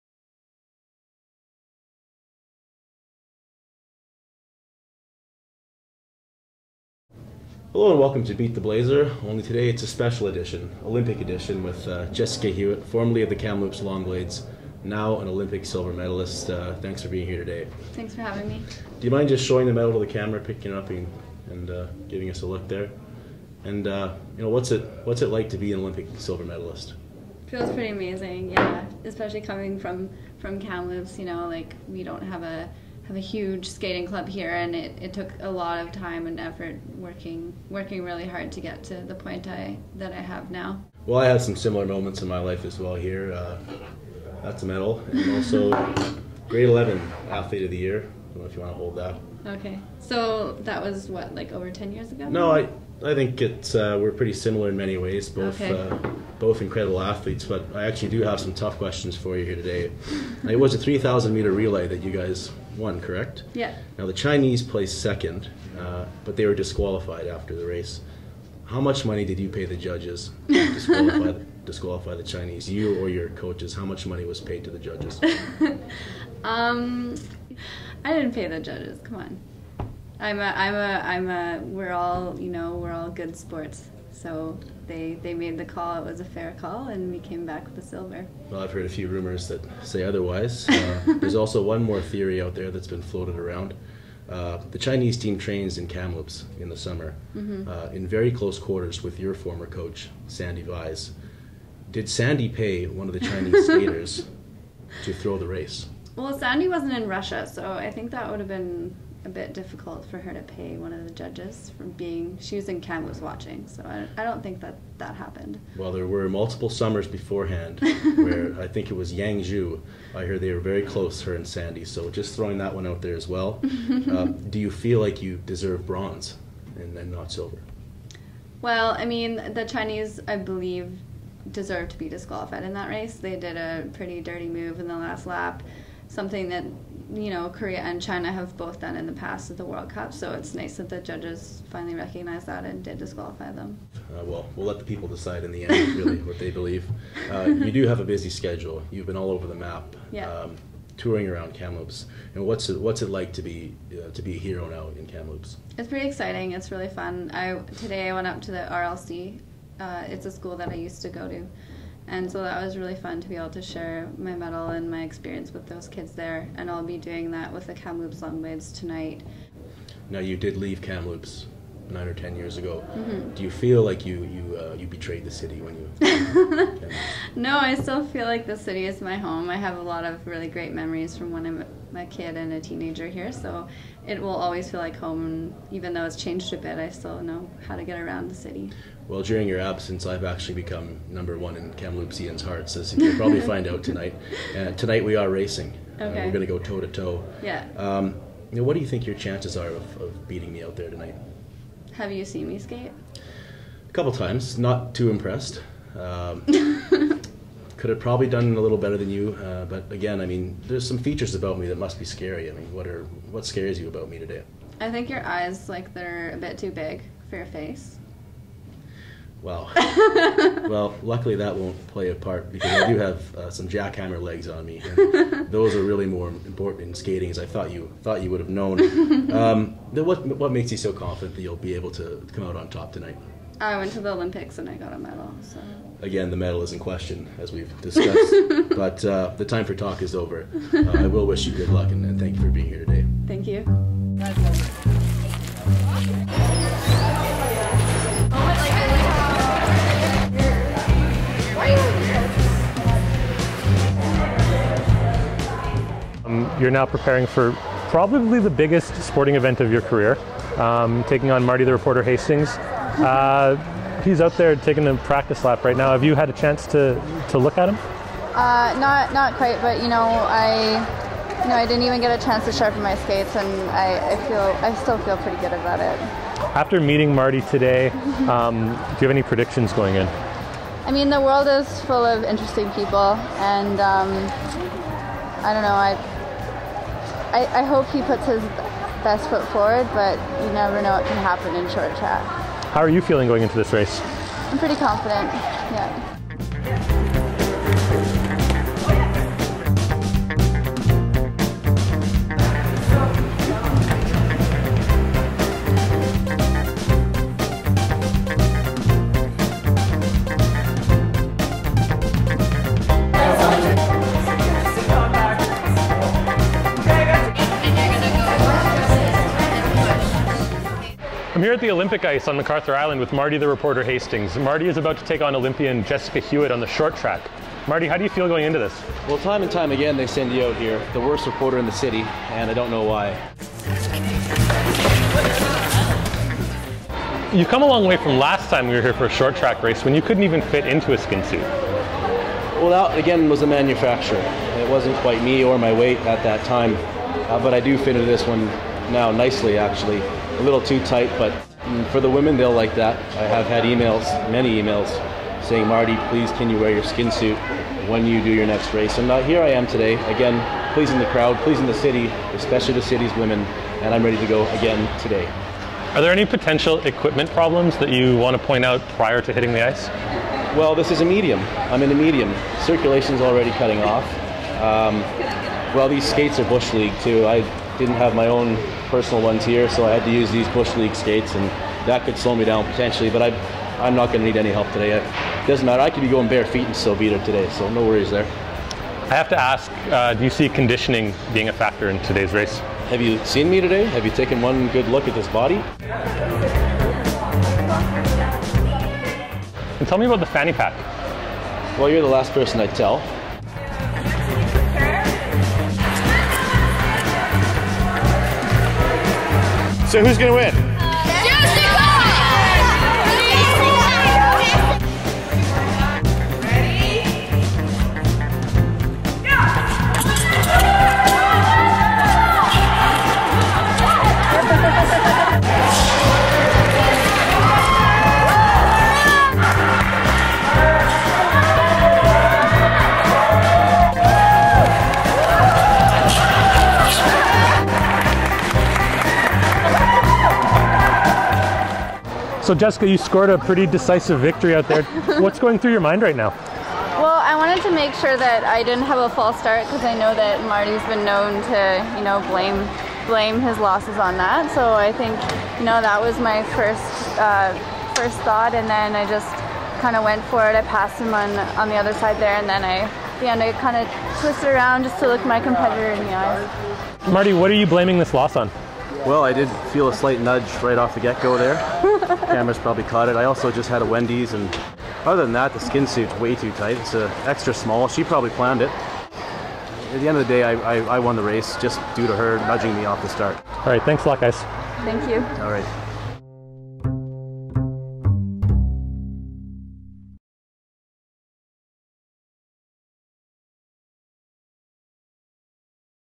Olympic silver medalist Jessica Hewitt vs Marty The Reporter
Marty The Reporter has long backed himself as the fastest skater in the Tournament Capital. When Jessica Hewitt, formerly of the Kamloops Long Blades, won silver at the Olympic Winter Games in Sochi, Russia,  Marty issued a challenge.